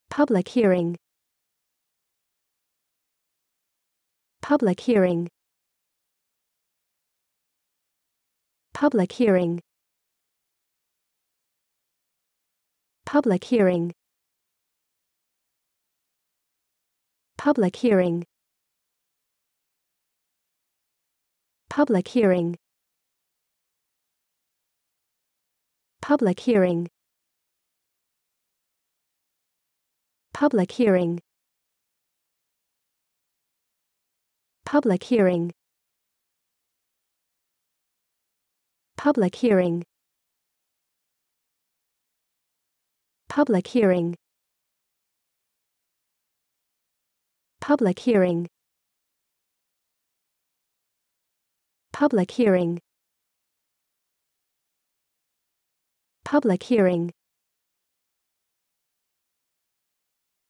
Como dizer 'audiências públicas' em ingles?
Como dizer audiências públicas em Ingles? Como se pronuncia public hearing? How to pronounce public hearing?